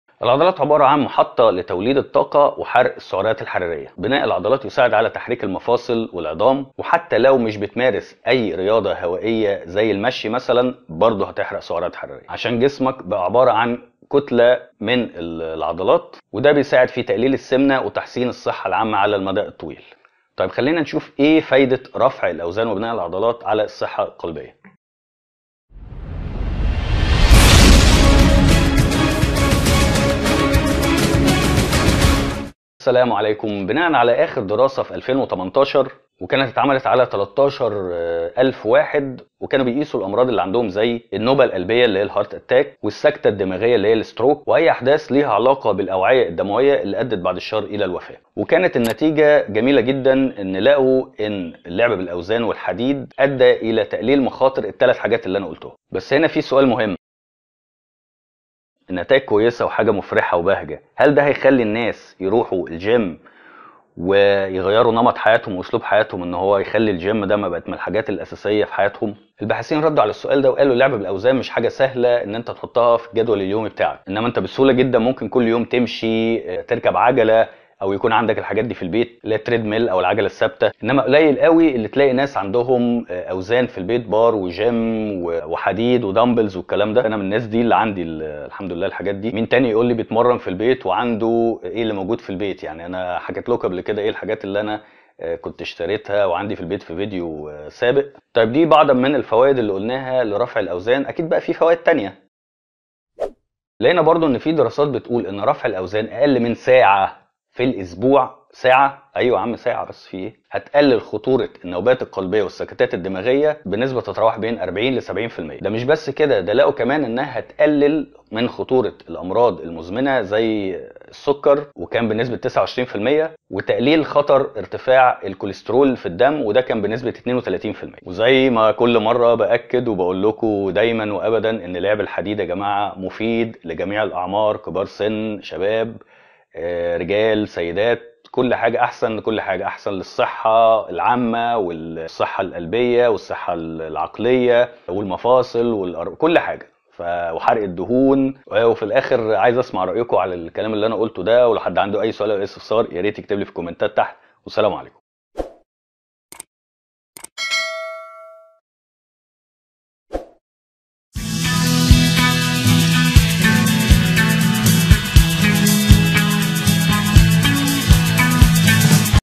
فوائد رياضة كمال الاجسام لصحتك القلبية والجسدية
ما فوائد رياضة كمال الاجسام ورفع الاوزان لصحتك القلبية والجسدية.

◄ لدعم القناة و الحصول علي عضوية VIP  مع الكابتن

← يمكنك تحميل كتيب كمال الاجسام مجانا ستجد فيه كل ما تريد معرفته عن كمال الاجسام و التغذية و التدريب..

√ Mohamed Shaalan
Certified Personal Trainer & Fitness Nutrition Specialist